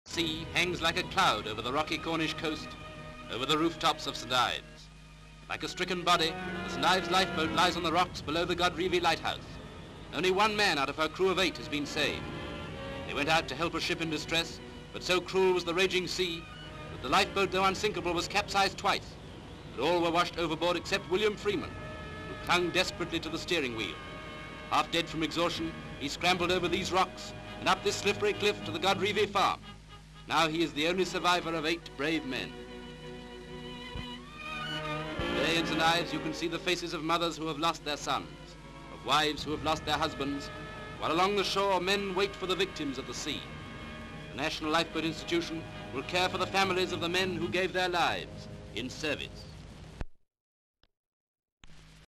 Decorating Us Coastguard (1939)
Oregon, United States of America.

L/S''s of Coastguards from the United States of America being decorated on board a large warship by senior naval officials. Very good L/S's of a lifeboat riding through very rough seas.

Note: The commentary with this item does not match the story, it appears to be from another story called 'Lifeboat Tragedy at St. Ives', Canister No. 39/8
 FILM ID:995.26